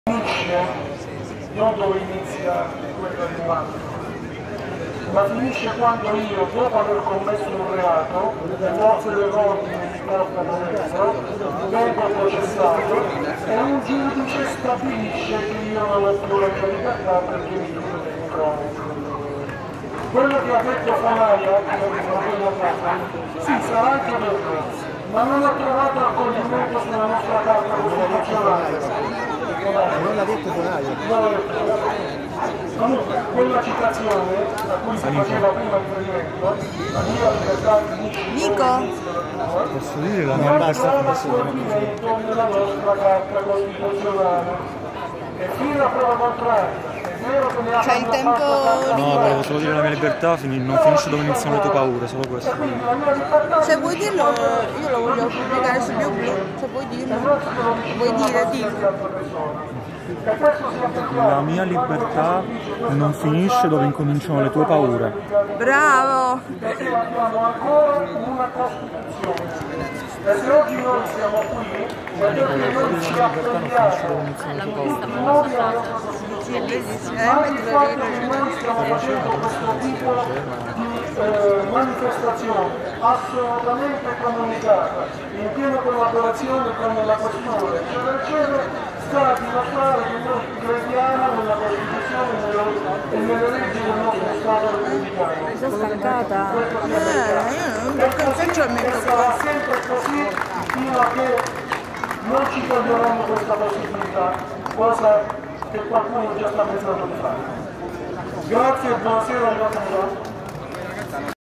NOGREENPASS Pescara 11.09.2021:'La mia libertà non finisce dove incominciano le tue paure'
NOGREENPASS PESCARA La mia libertà finisce dove inizia la tua?
La parola ad un ragazzo che per problemi di tempo relativi ai permessi comunati di svolgimento della manifestazione non ha potuto esprimere il suo concetto: NOGREENPASS PESCARA
'La mia libertà  non finisce dove incominciano le tue paure'

NOGREENPASS Pescara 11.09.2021: 'My freedom does not end where your fears begin'